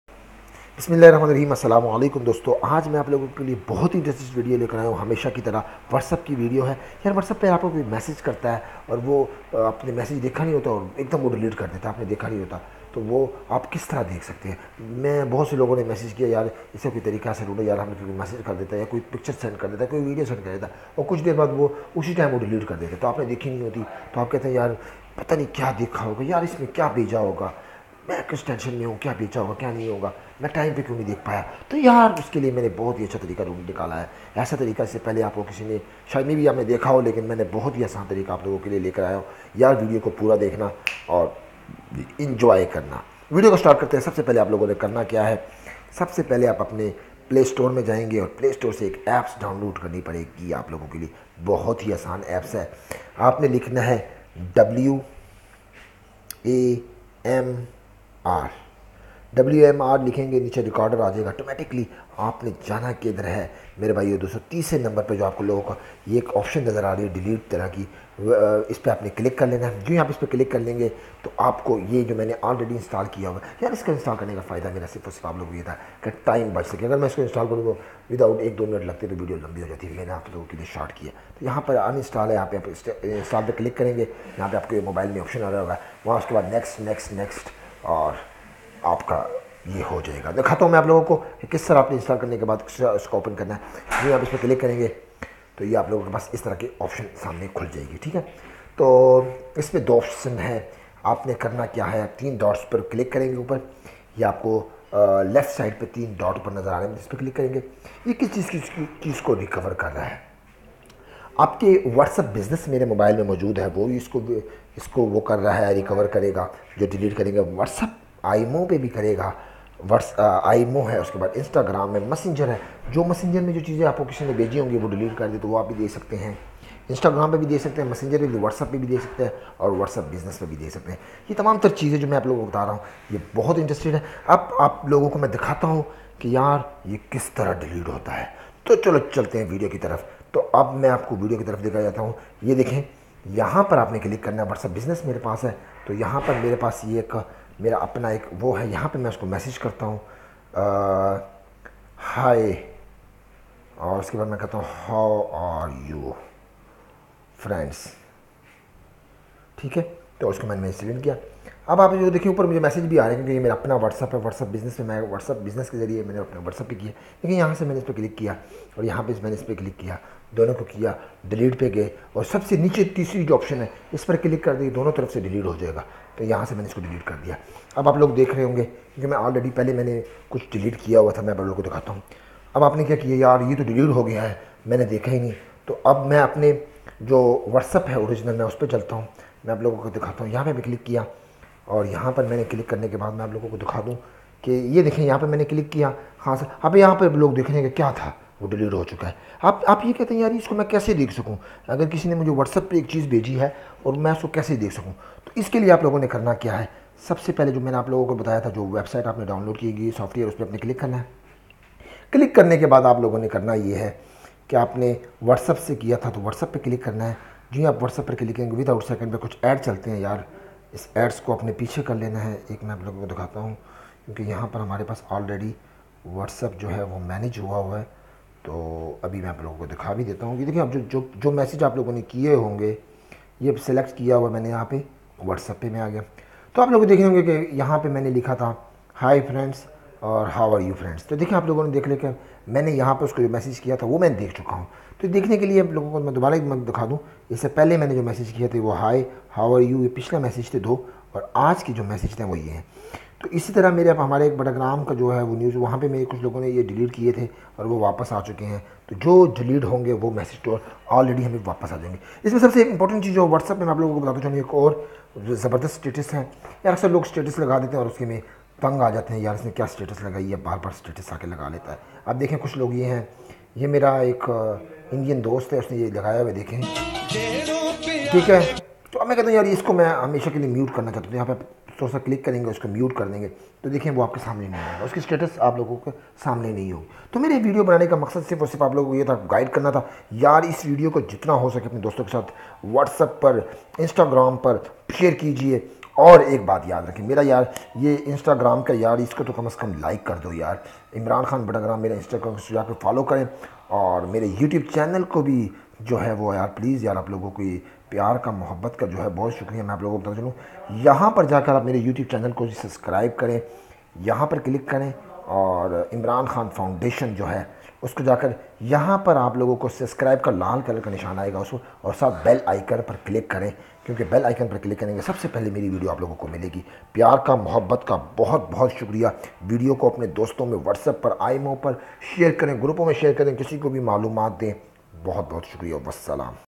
On WhatsApp, you will have a message and delete it, then you can see it again with an easy way
یار یہ طریقہ اس سے پہلے کسی نے نہیں بتایا ہوگا اگر ویڈیو اچھی لگے تو دوستوں سے شیئر کیجیے لائک کیجئے کمنٹ کیجئے بہت بہت شکریہ